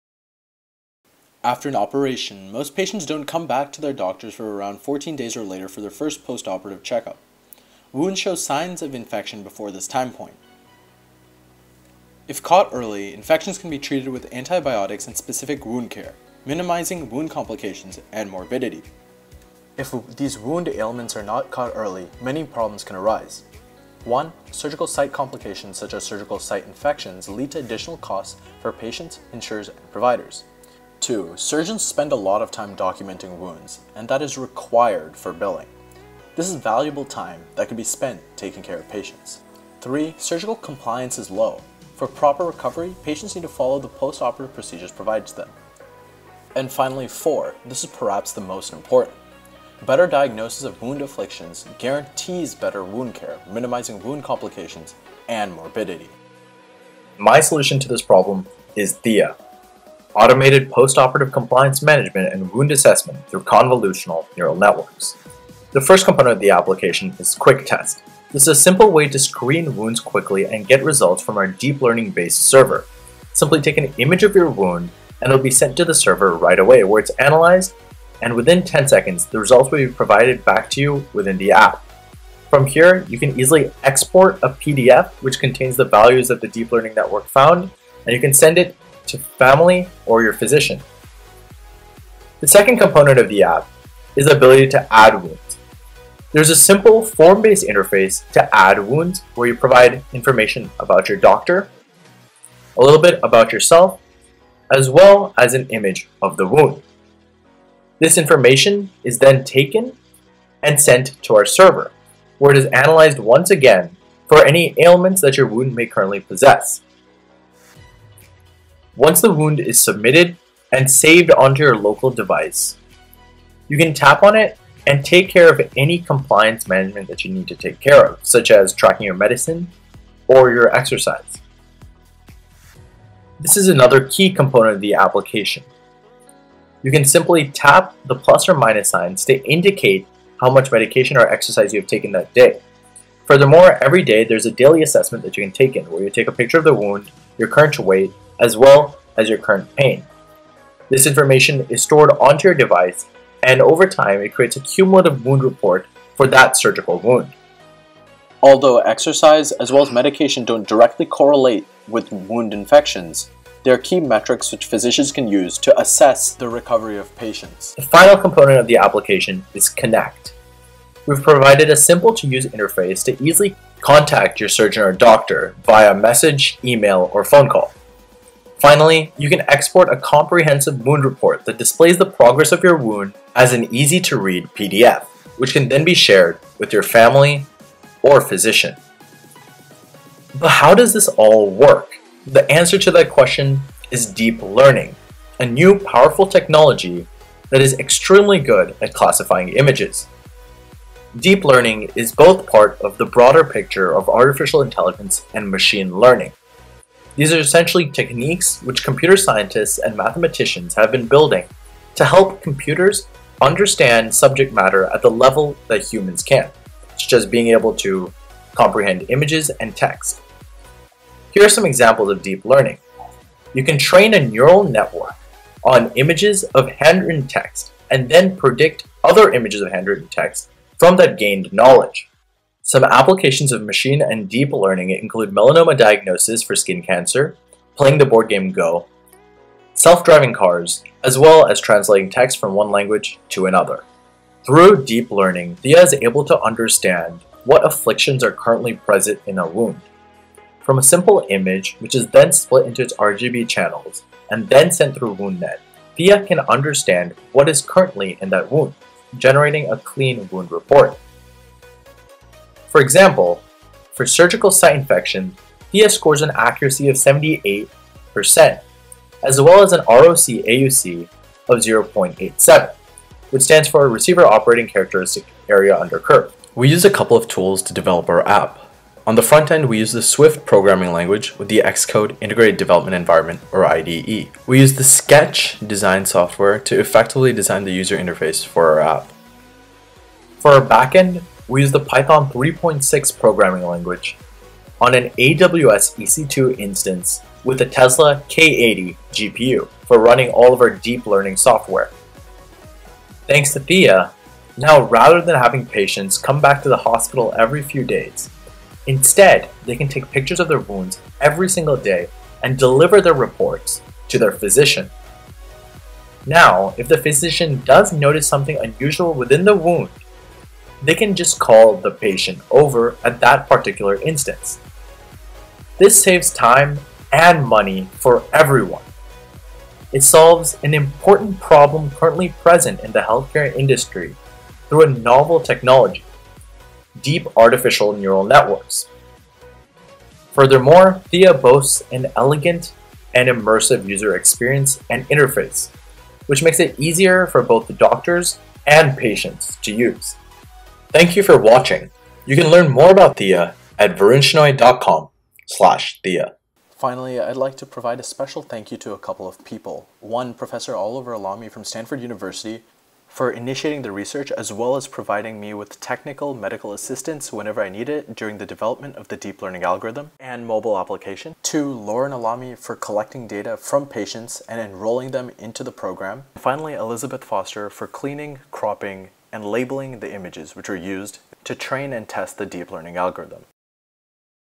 Theia App Demo
Deepwound: Automated Postoperative Wound Assessment and Surgical Site Surveillance through Convolutional Neural Networks